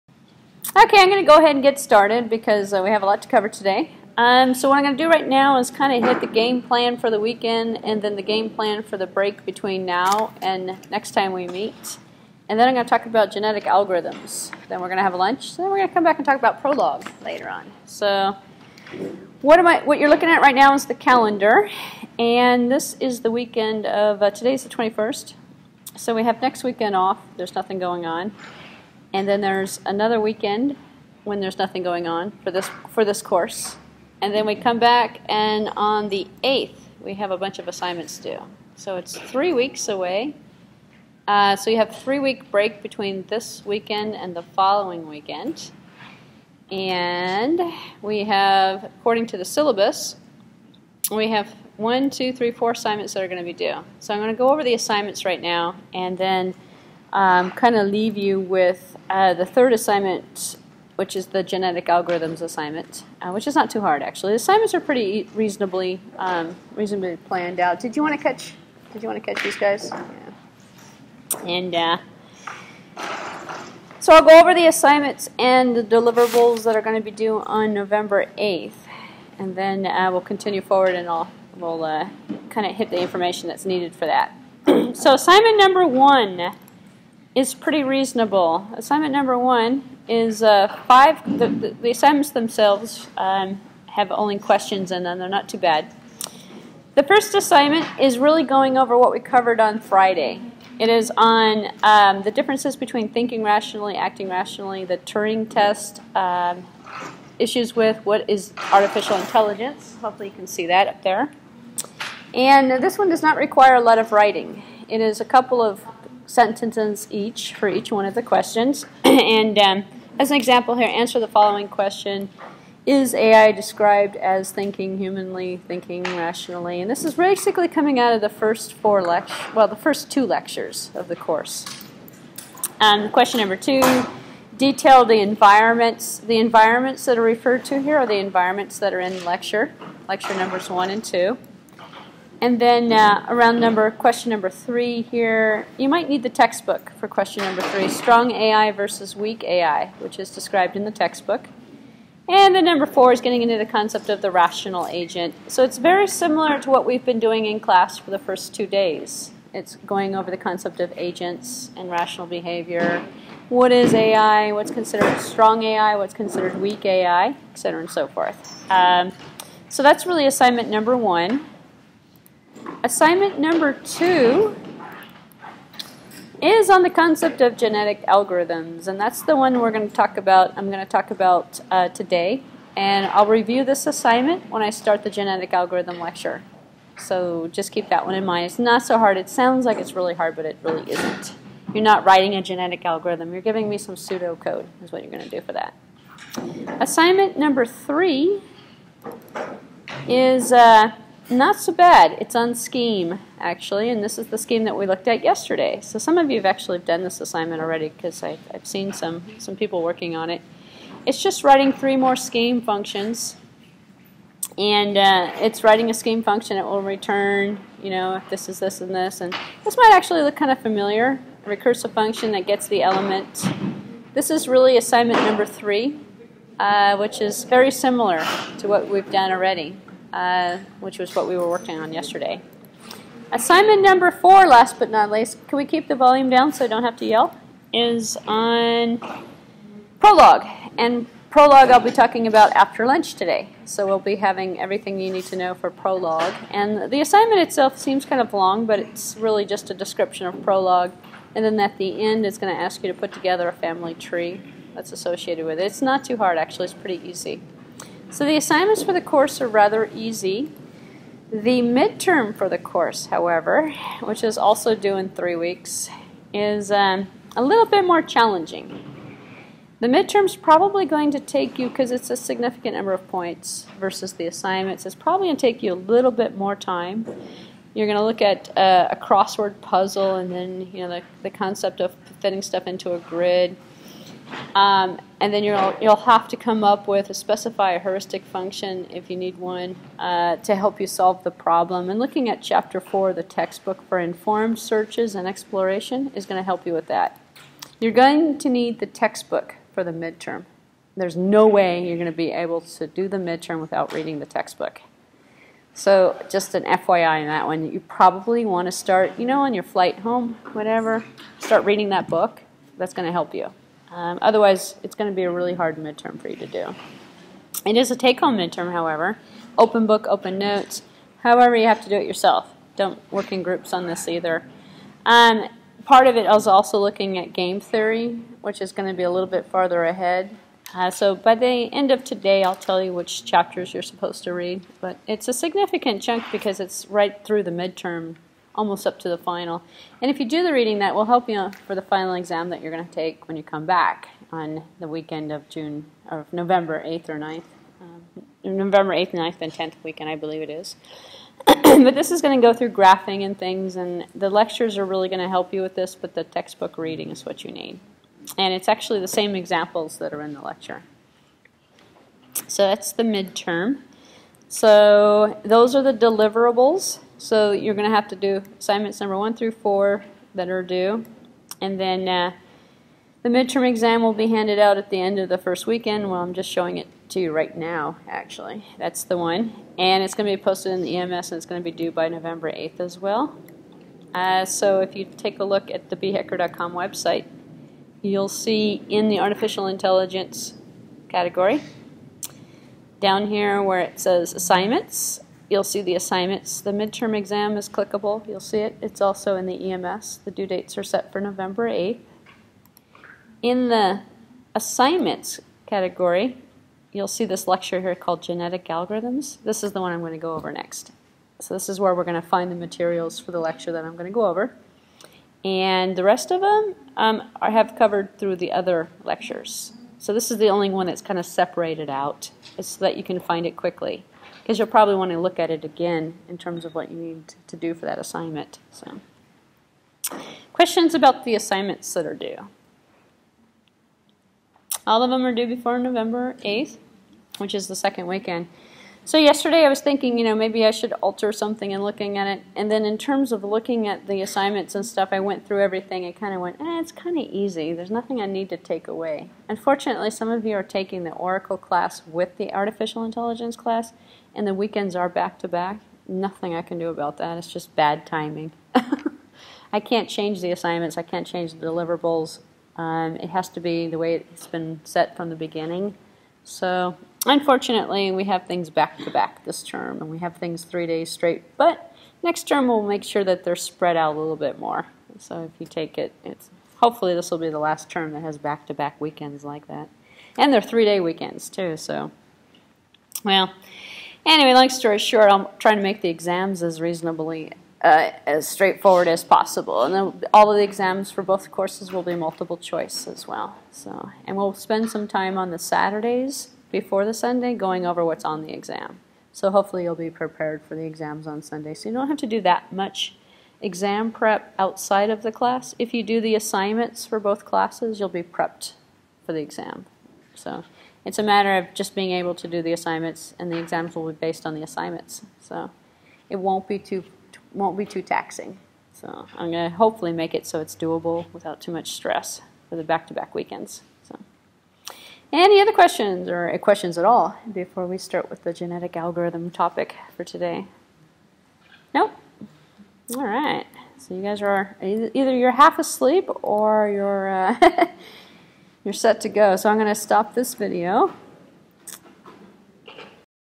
Artificial Intelligence Take-Home Assignments 1-4
This lecture covers how to complete the first four take-home assignment for the weekend AI class. General YouTube viewers are not going to find this video helpful. It is intended for students taking the weekend AI class only.